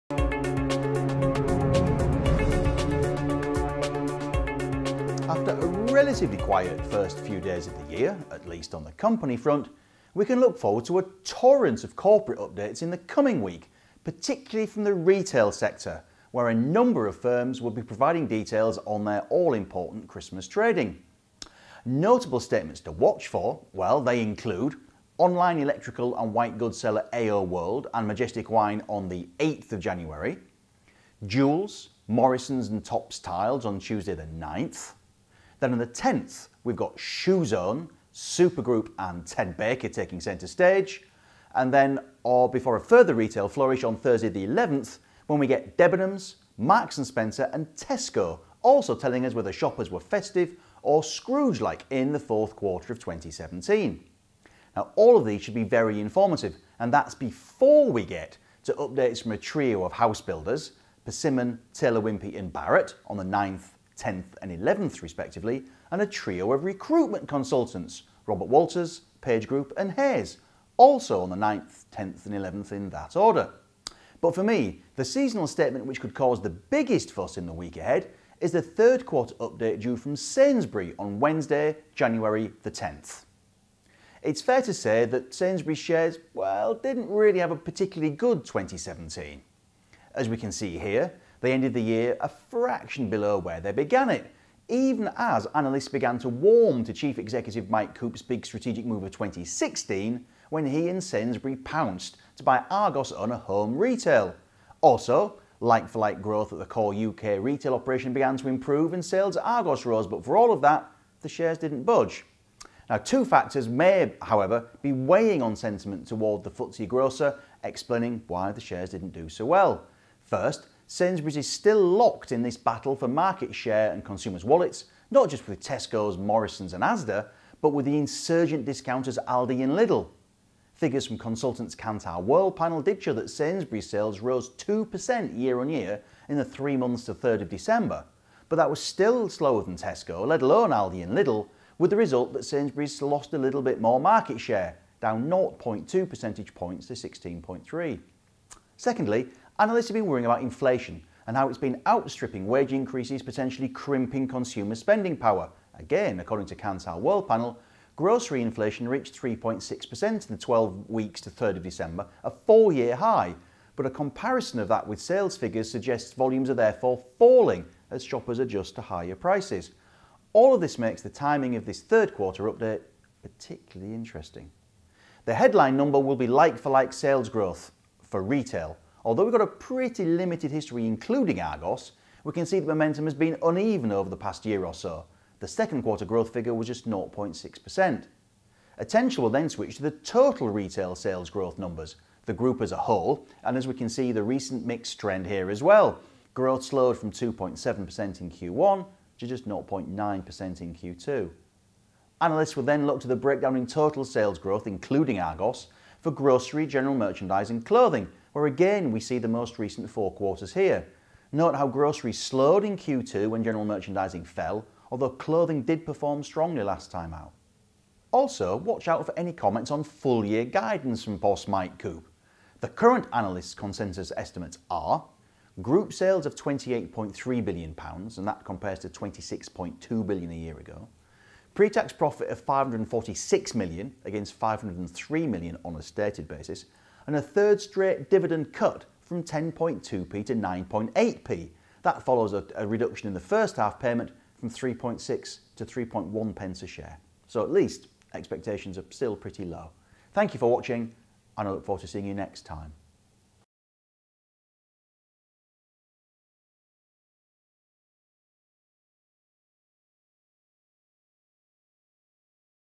AJ Bell Youinvest Breaking the Mould - Sainsbury's trading update
Russ Mould, AJ Bell Investment Director, discusses next week's company reports, including Sainsbury's third quarter trading update.